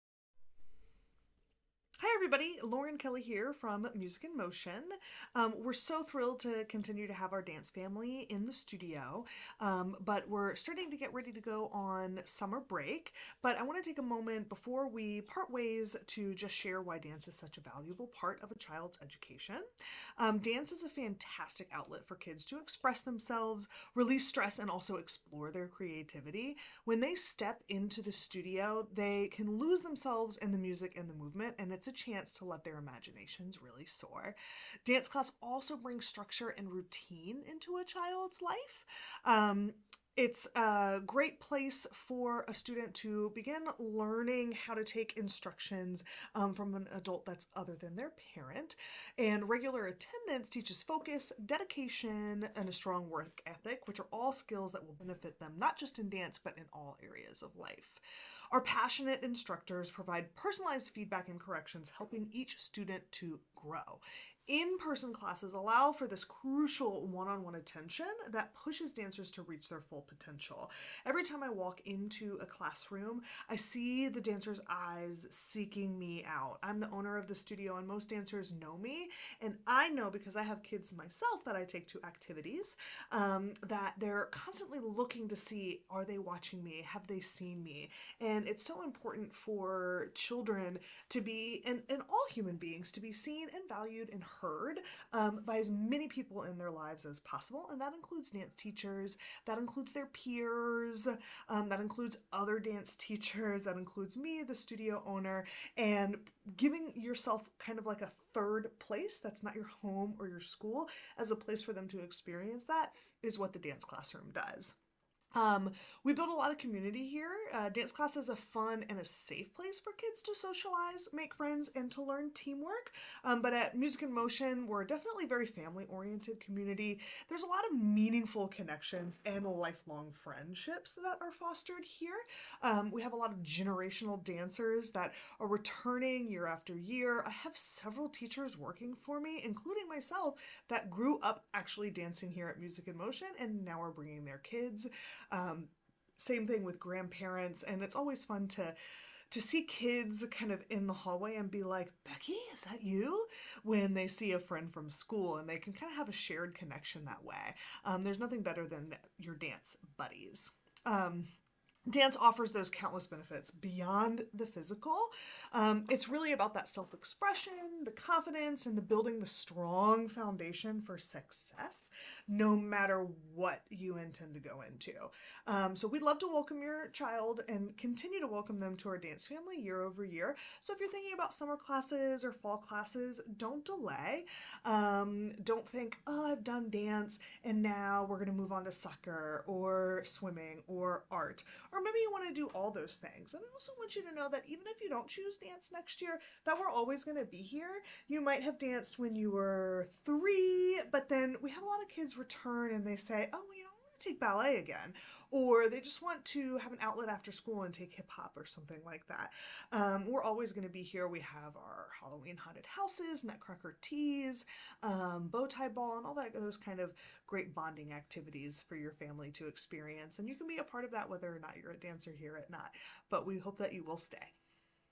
Benefits of Dance in the Classroom
Why Dance Belongs in the Classroom

Dance isn't just about learning steps! This video explores the many reasons why dance is a valuable part of a child's education, fostering creativity, confidence, teamwork, and a strong work ethic.